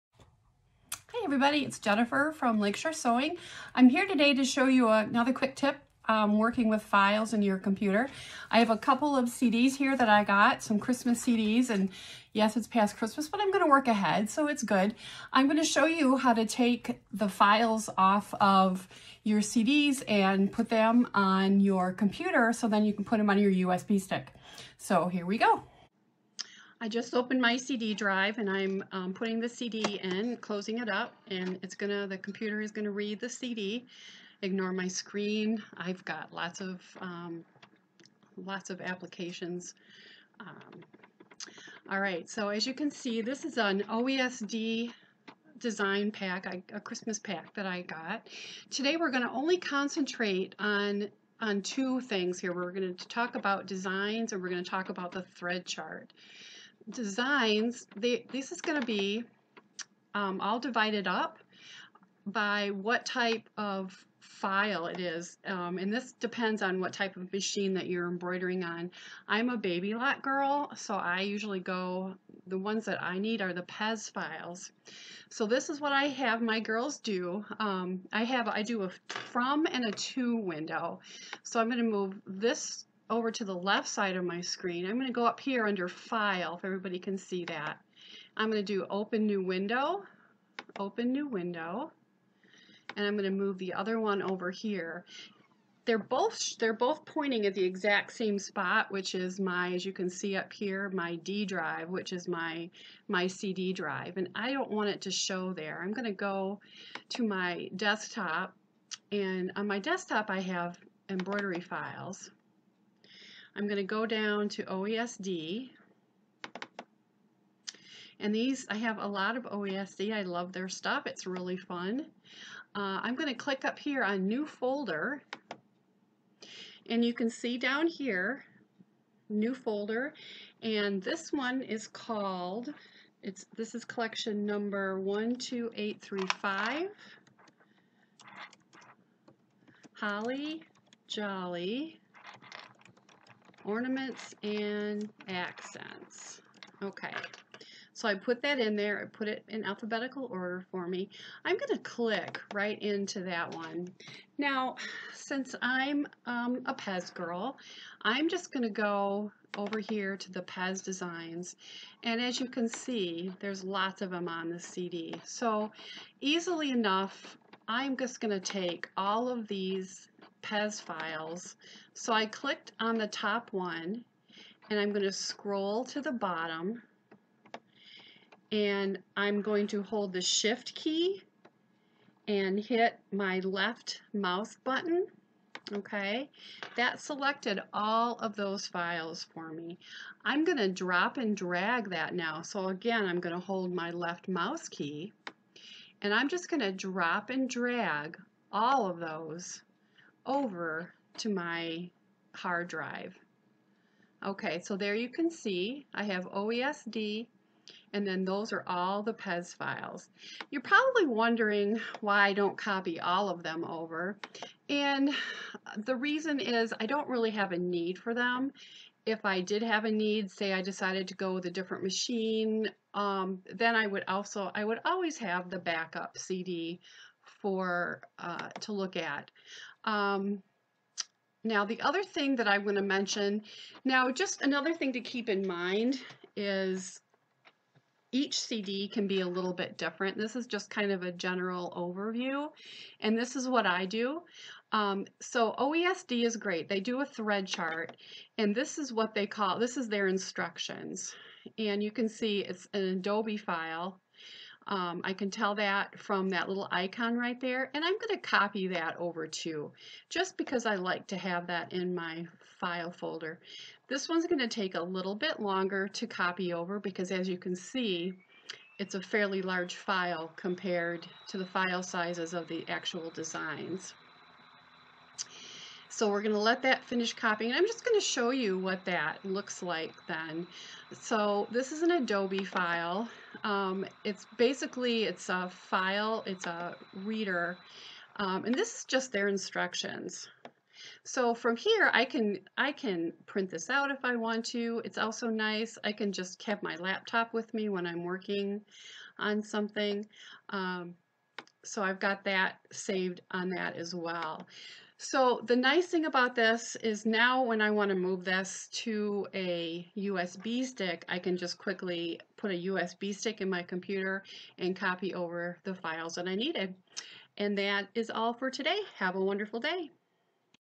CD to Computer Video
Quick demo on how to move files from a CD to your computer.